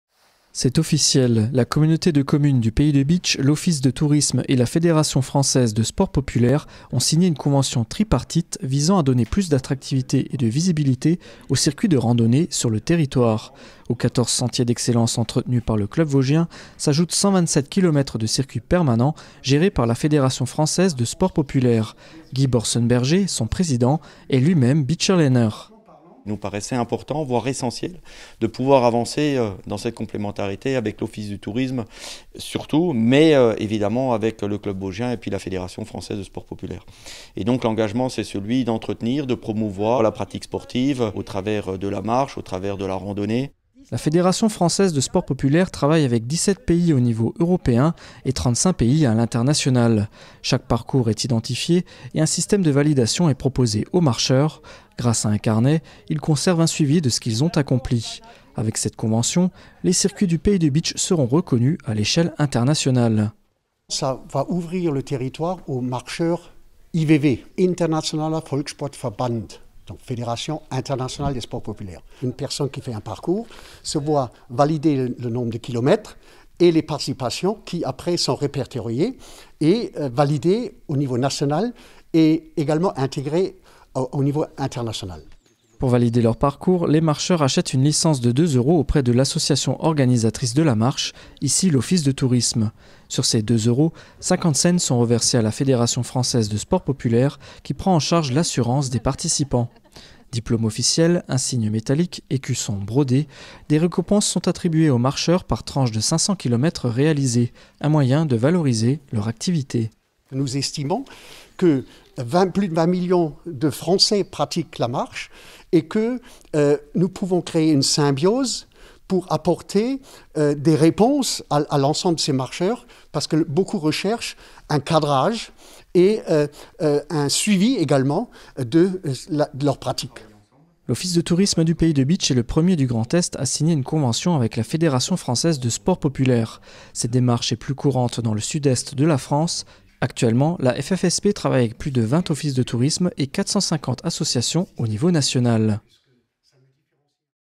Un pas de plus pour la randonnée au Pays de Bitche
La CCPB, l’Office de Tourisme et la Fédération Française de Sports Populaires ont signé une convention tripartite visant à attirer plus de marcheurs sur le territoire.
Réalisation : David HORÈS